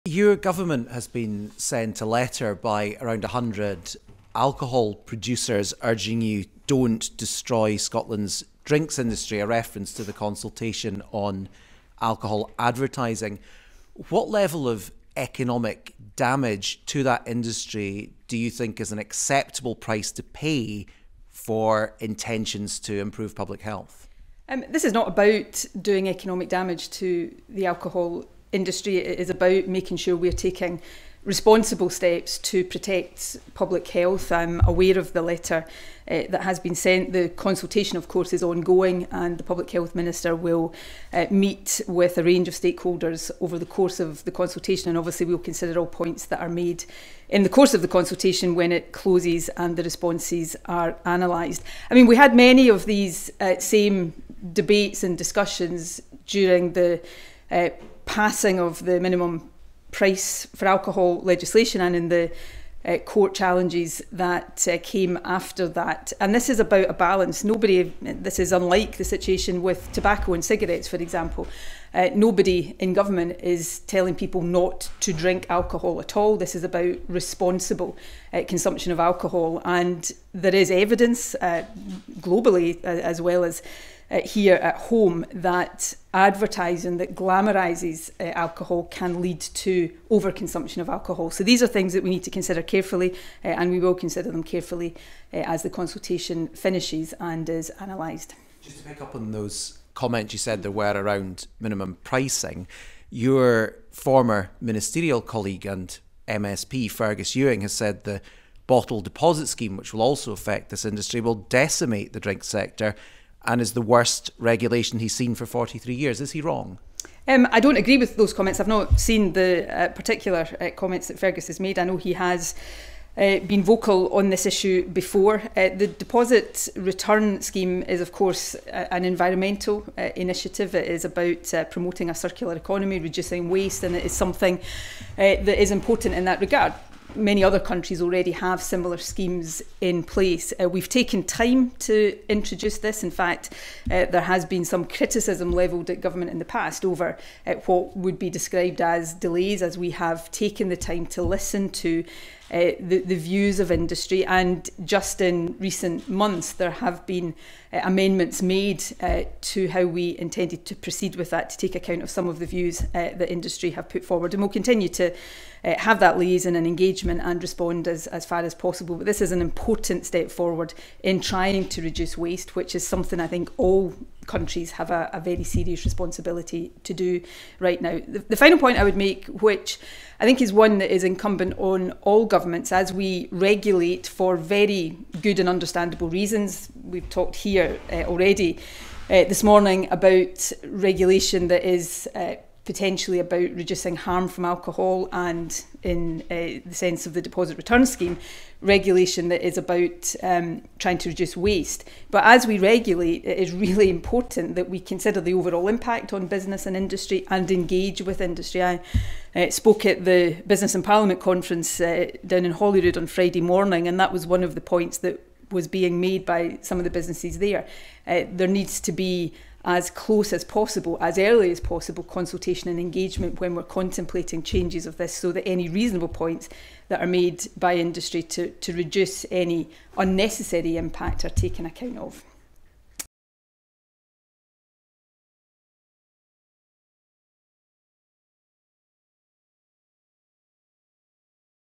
Nicola Sturgeon doesn't think advertising ban will economically damage alcohol industry in Scotland
Today Nicola Sturgeon was asked about a letter that was sent to the Scottish government from alcohol producers across Scotland saying 'don't destroy Scotland's drinks industry'

The companies want a proposed ban on alcohol advertising scrapped. In it's most extreme cases would stop alcohol branding or advertising in public spaces or online